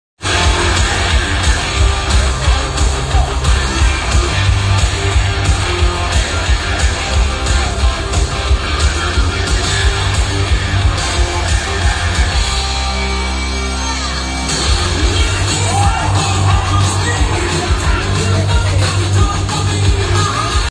For the Love of Horror Cosplay Competition
12/11/23 Bowlers Manchester. No cosplay competition is complete without an air guitar battle.